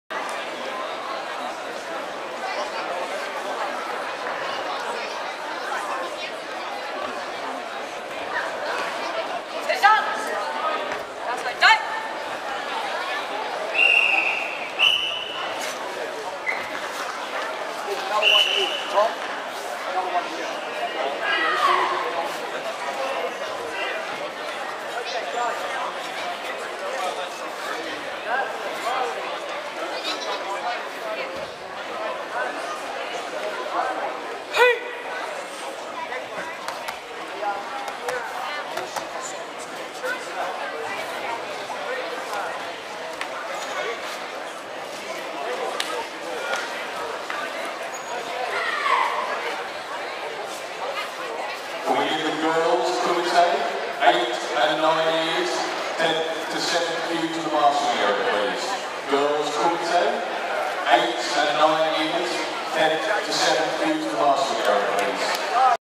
Connor Bassai Dai
This is a clip from the 2014 Shotokan Cup held at the Melbourne Sports and Aquatic Centre, 1 Aughtie Drive, Albert Park VIC 3206. The Karate School [Genseiryu] had a record number of students competing, most in their first ever tournament. Mauro Sensei was very proud of all their performances as everyone tried their best, even when they were a little nervous and unsure of what was happening.

Our School did incredibly well with numerous Gold, Silver and Bronze placing's. Everyone should be proud of their efforts and participation. Thanks go to all the parents and helpers who made our schools participation happen.